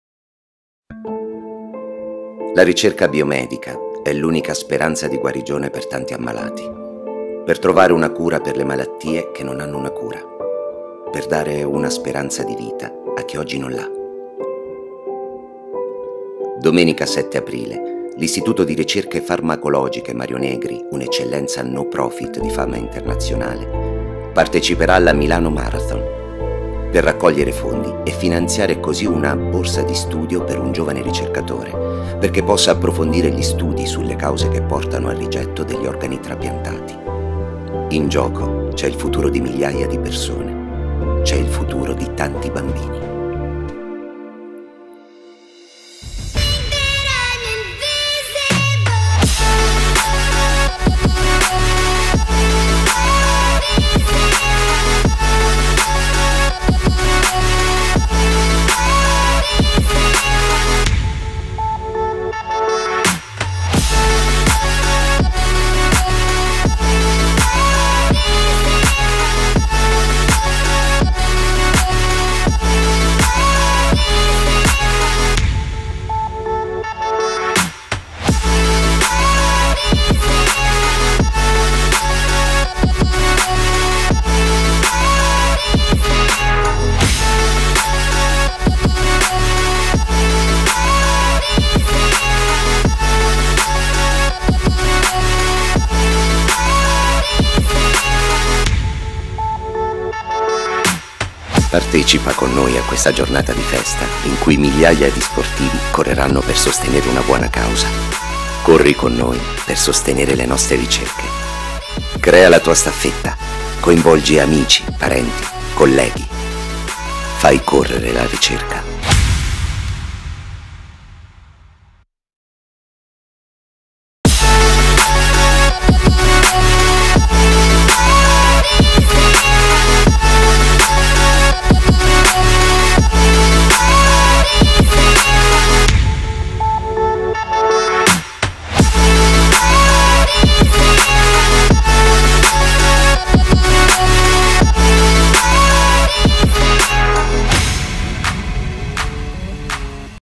L'Istituto Mario Negri alla Milano City Marathon 2019
Domenica 7 aprile 2019 l’Istituto Mario Negri parteciperà per l’ottavo anno alla Milano City Marathon, aderendo al Charity Program, l’evento che affianca alla sfida sportiva l’impegno solidale. I runners corrono a sostegno di “una buona causa”: raccogliere fondi per una borsa di studio per un ricercatore impegnato in studi sui trapianti di organo e come migliorare l'efficacia dei farmaci anti-rigetto.
faicorrerelaricerca partecipando come staffetta dell'Istituto o sostenendo il nostro progetto con una donazione.